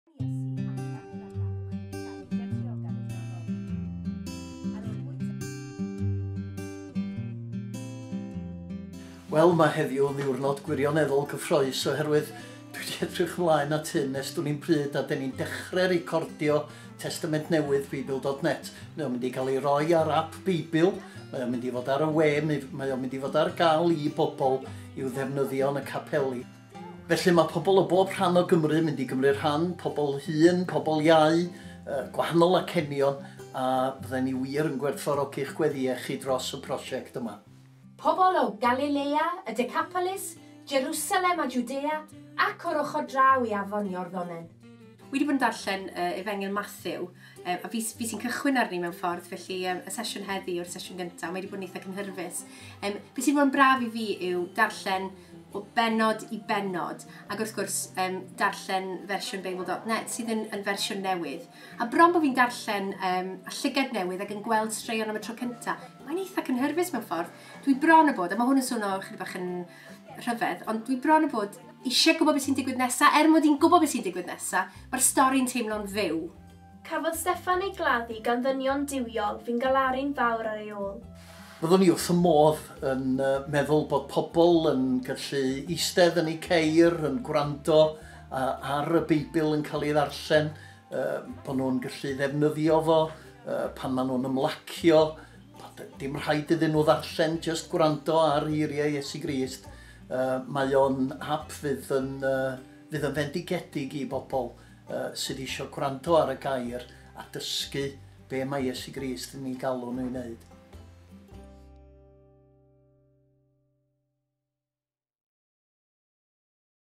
lleisio beibl.net
Dyma ddechrau ar brosiect mawr o recordio testun beibl.net ar lafar.  mi fydd y cyfan yn cael ei osod ar Ap Beibl maes o law. // The beginning of an exciting project to record the New Testament using the modern translation of the Bible; beibl.net